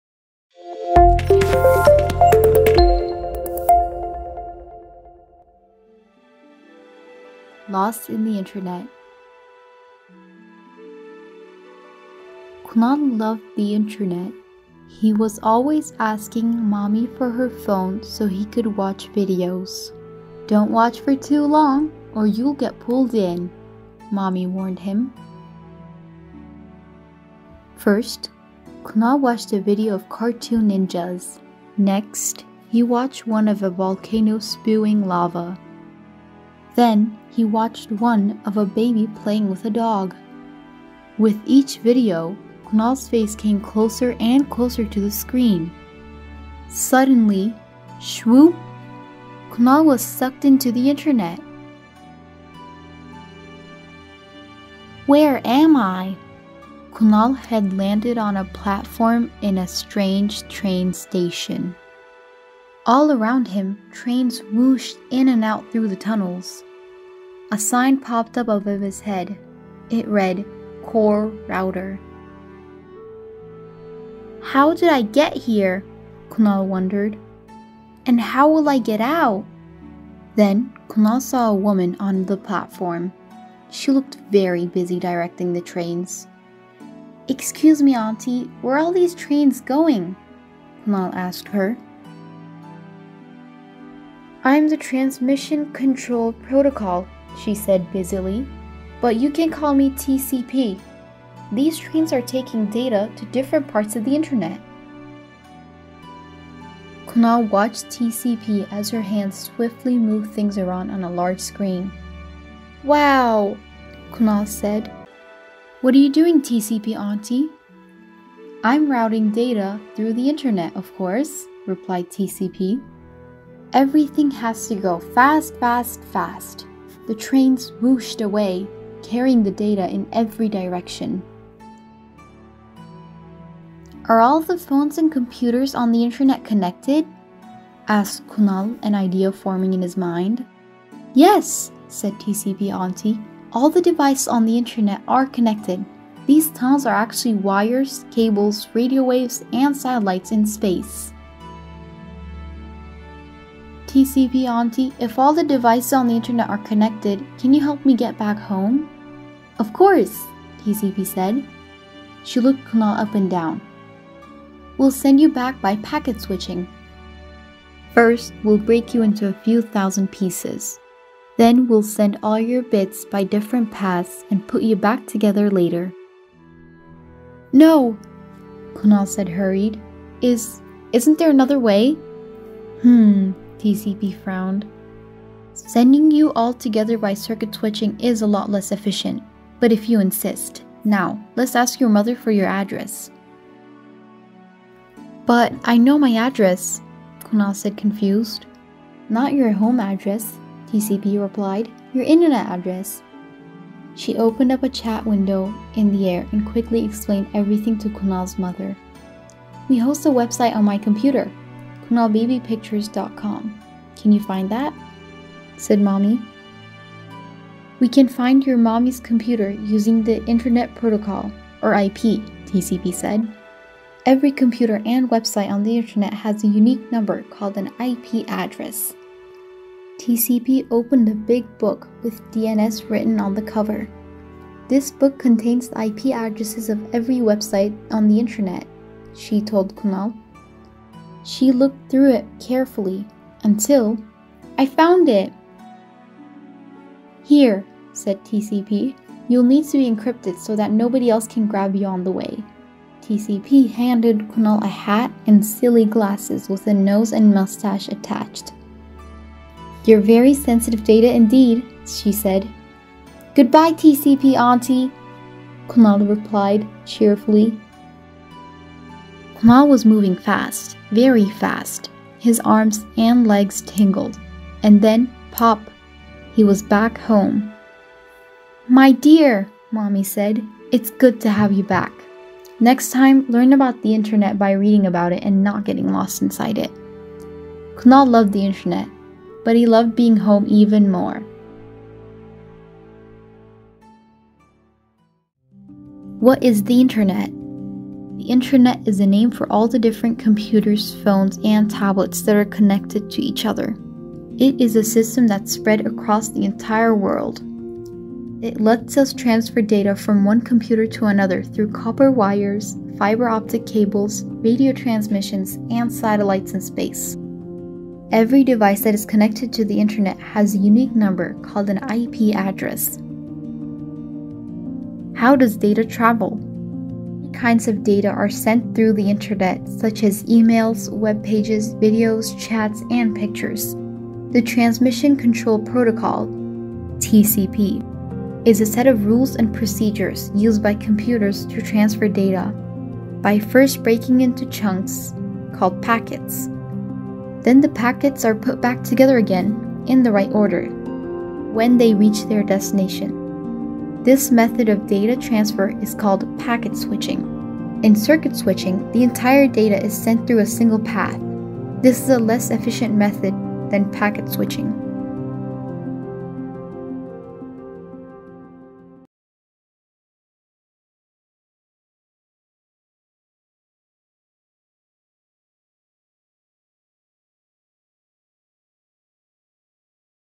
Lost in the Internet - Story for Kids
Everyday we use the Internet but do you know how it works?
Let's learn it all together with Kunal in this story.

Timestamps
00:00 - Intro
00:06 - Lost in the Internet Story
04:55 - What is the Internet?
05:25 - How Does Data Travel?